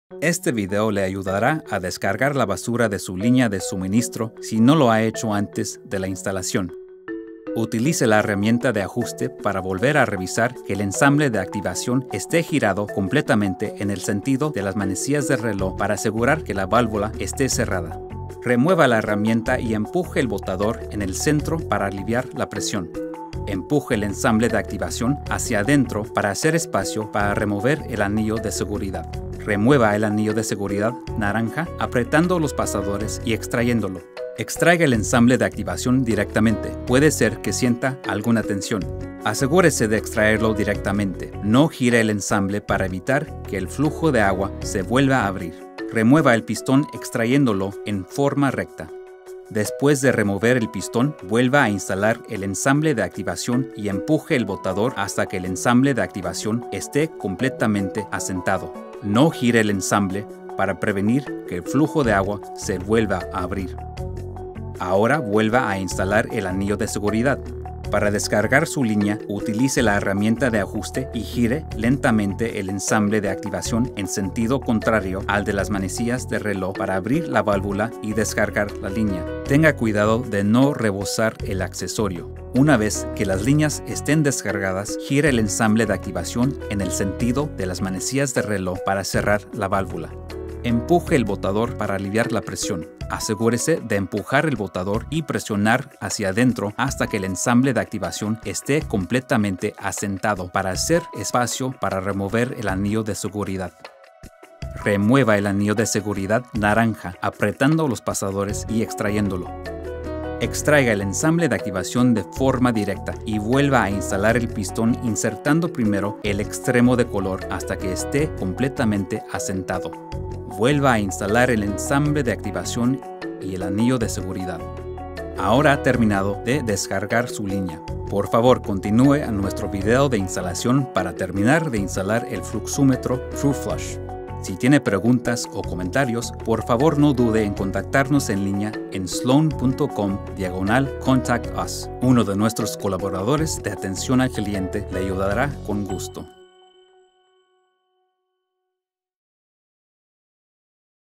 Fluxómetro Sloan Truflush
Disponible con operación manual o por sensor, el fluxómetro Sloan TruFlush satisface las necesidades del mercado global a medida que la conservación del agua se vuelve aún más crítica en la actualidad. Disponible en una variedad de acabados especiales.

PRODUCTOS MOSTRADOS:
Fluxómetro manual oculto para sanitario TRF 156 TruFlush

TRF 8156 Fluxómetro para Sanitario con Sensor Oculto TruFlush

LEER FOLLETO:
Folleto de Fluxómetro TruFlush